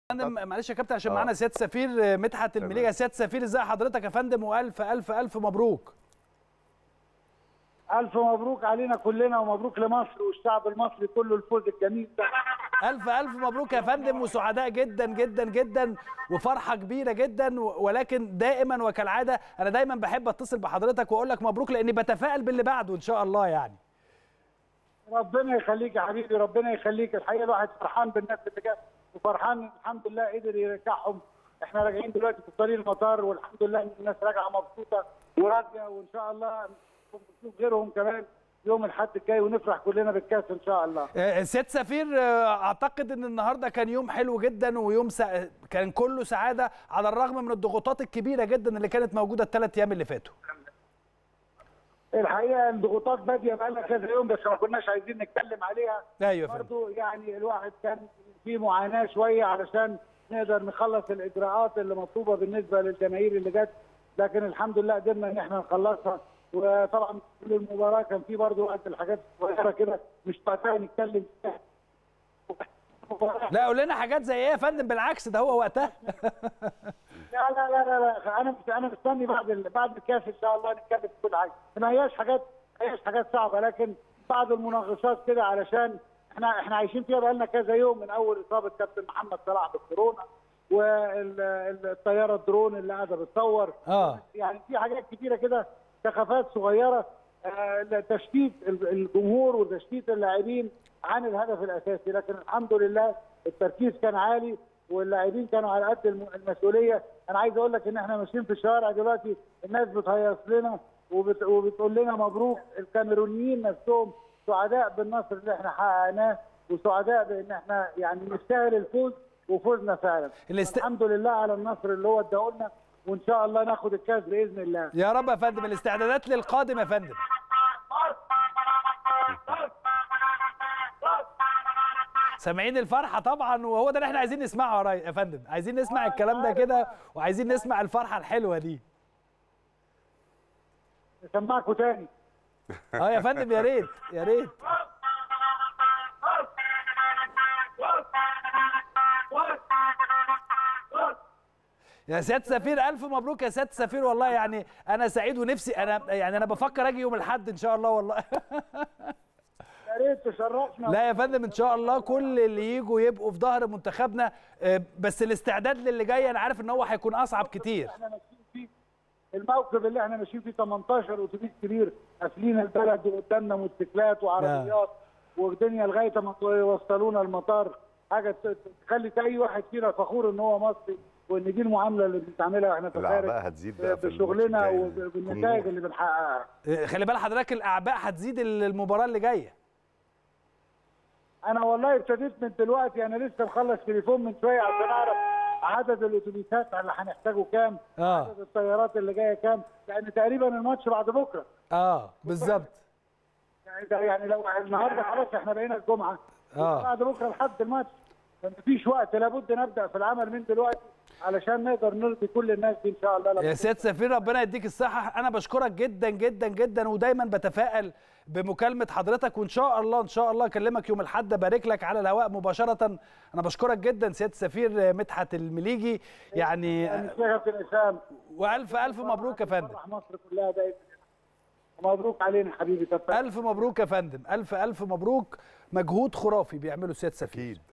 سفير مصر بالكاميرون: تعرضنا لسخافات وضغوطات والحمد لله التركيز كان عالي واللاعبين على قدر المسئولية
سفير مصر في الكاميرون : تعرضنا لسخافات وضغوطات والحمد لله التركيز كان عالي واللاعبين كانوا على قدر المسئولية